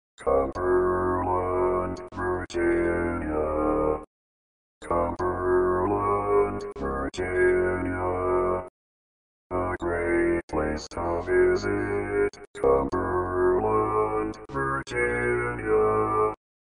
How to Say or Pronounce USA Cities — Cumberland, Virginia
This video shows you how to say or pronounce Cumberland, Virginia.

A computer said Cumberland, Virginia. How would you say Cumberland, Virginia?